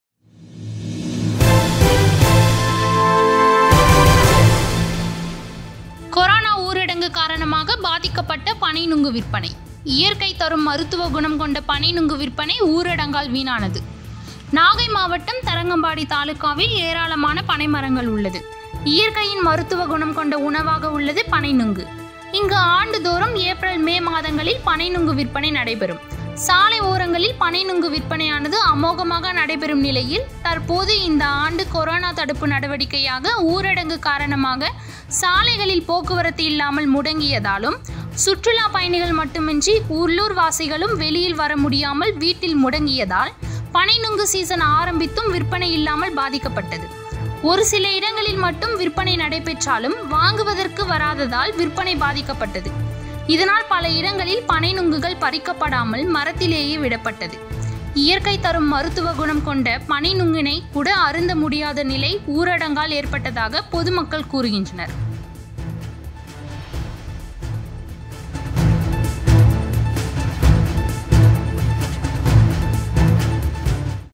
நாகை மாவட்டத்தில் பாதிக்க பட்ட பண்ணை நொங்கு விரவனை | Aram News Tamil
கொரானா ஊரடங்கு காரணமாக பாதிக்கப்பட்ட பனை நுங்கு விற்பனை, இயற்கை தரும் மருத்துவ குணம் கொண்ட பனை நுங்கு விற்பனை ஊரடங்கால் வீணானது...